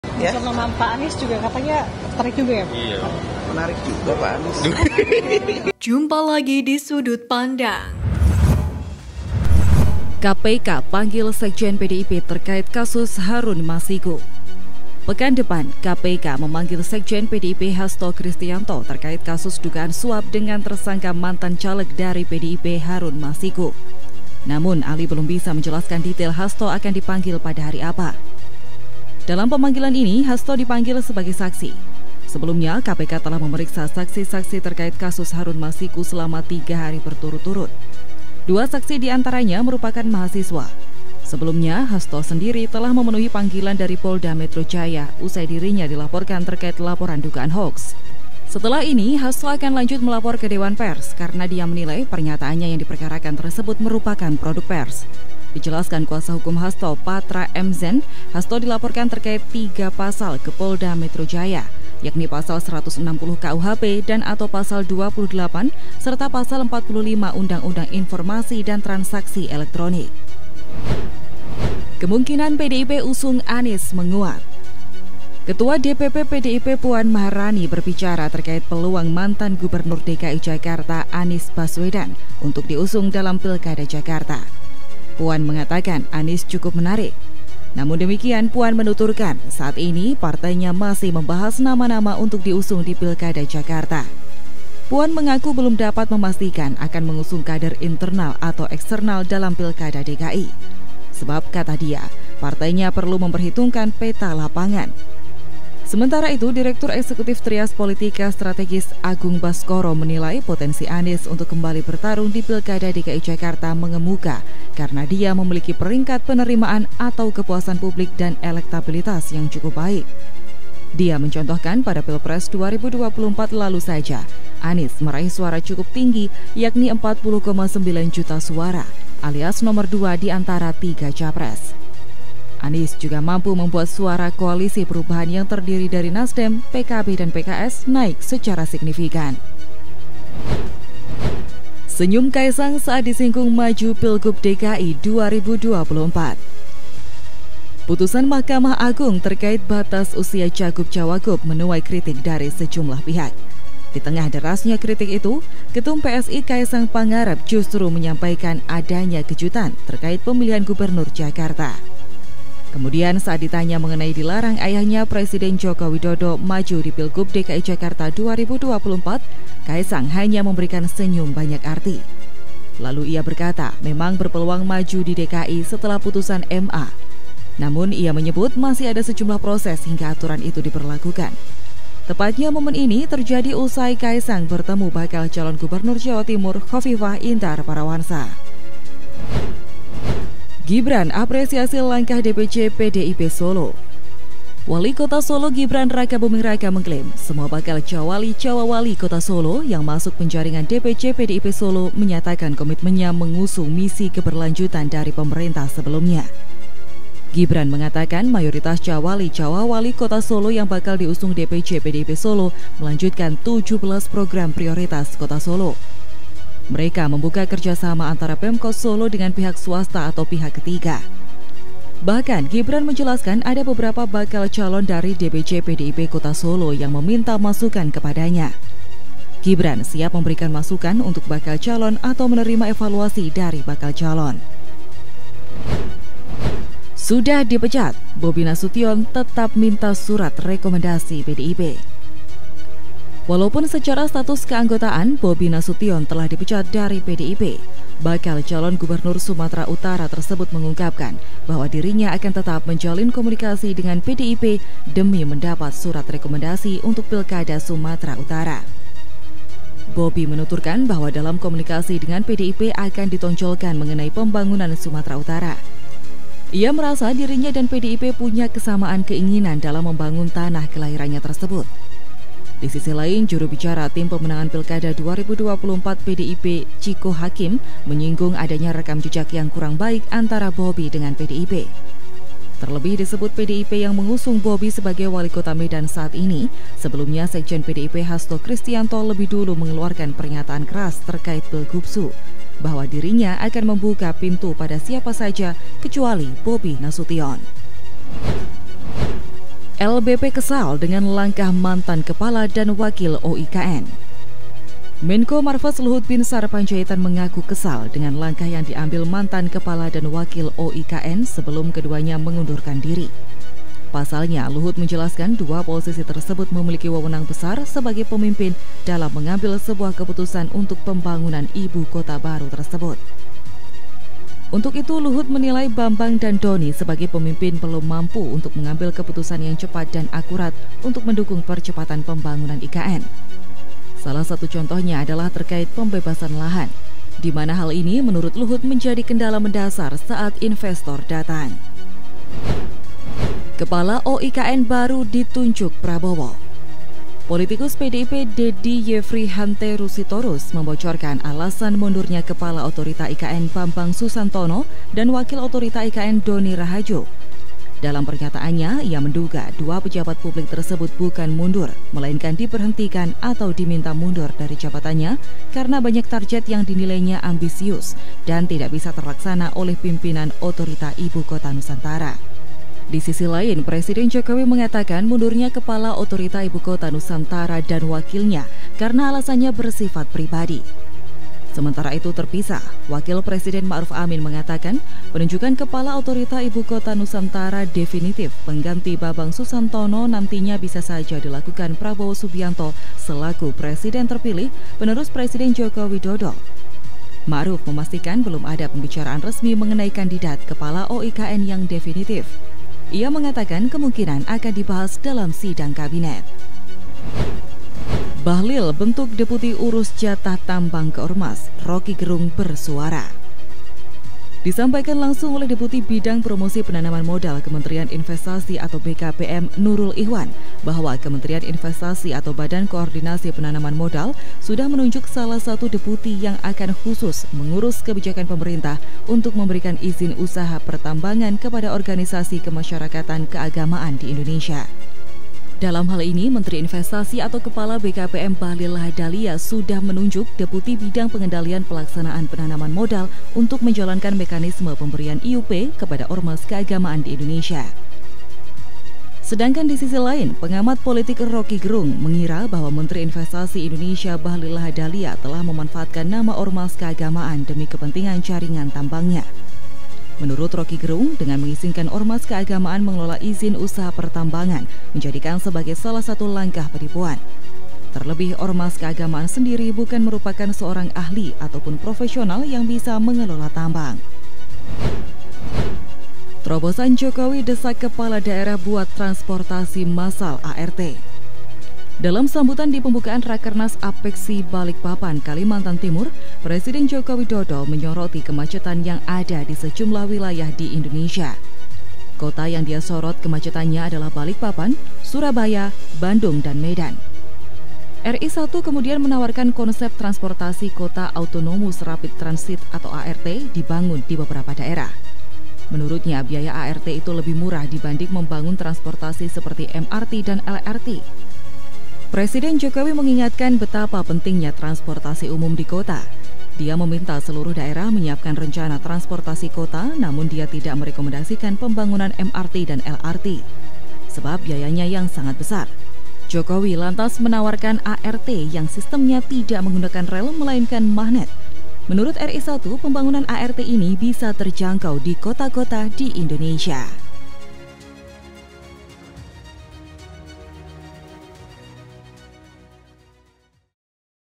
GEGER! AKHIRNYA TERBONGKAR, KPK Panggil Sekjen PDIP~Luhut Luapkan KEKESALAN~Jokowi Ambil Trobosan
BERITA TERBARU INDONESIA HARI INI, SERTA UPDATE TERKINI DUNIA POLITIK - Berita Sudut Pandang Hari Ini : GEGER! AKHIRNYA TERBONGKAR, KPK Panggil Sekjen PDIP~Luhut Luapkan KEKESALAN~Jokowi Ambil Trobosan

00:00 - OPENING
00:09 - KPK PANGGIL SEKJEN PDIP TERKAIT KASUS HARUN MASIKU
01:10 - KEMUNGKINAN PDIP USUNG ANIES MENGUAT
02:20 - SENYUM KAESANG SAAT DISINGGUNG MAJU PILGUB DKI 2024
03:09 - GIBRAN APRESISASI LANGKAH DPC PDIP SOLO
03:55 - SUDAH DIPECAT, BOBY NASUTION TETAP.MINTA SURAT REKOMENDASI PDIP
04:58 - LBP KESAL DENGAN LANGKAH MANTAN KEPALA DAN WAKIL OTORITA IKN
05:42 - KEPALA OIKAN BARU DITUNJUK PRABOWO
06:46 - BAHLIL BENTUK DEPUTI URUS JATAH TAMBANG KE ORMAS, ROKCY GERUNG  BERSUARA
07:54 - TROBOSAN JOKOWI DESAK KEPALA DAERAH BUAT TRANSPORTASI MASAL ART
Sumber :